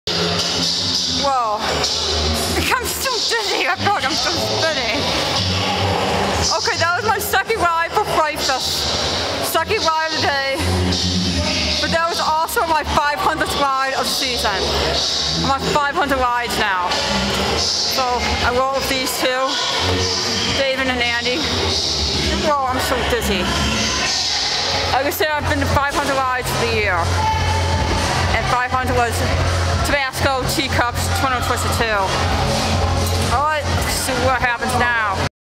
My 500th ride on Chubasco
I let David pick what would be my 500 ride of the season. He pick the Chubasco tea cups. I rowed my 500 ride with David & Andy.